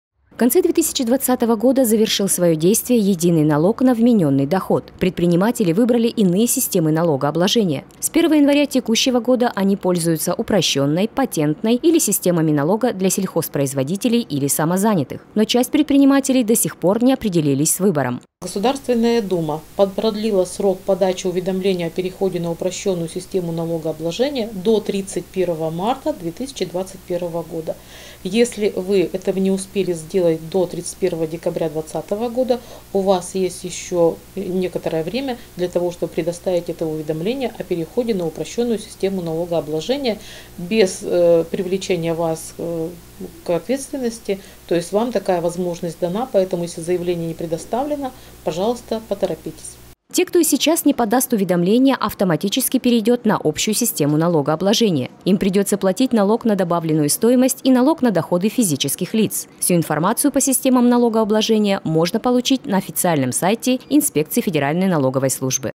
Выбор режима налогообложения продлили
С 1 января 2021 года единый налог на вмененный доход прекратил свое существование. Организации и индивидуальные предприниматели выбрали иные налоговые режимы. У тех, кто не определился с выбором остался последний шанс.